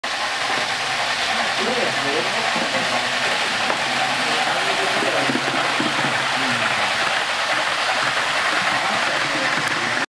EC172 / 173 "Vindobona"
EC "Vindobona" train, which links Wien and Hamburg via Praha and Berlin.  This train is composed with OBB, CD and DBAG coaches.  Electric locomotives is Rh1216 and this loco belongs to Austrian Federal Railways, OeBB.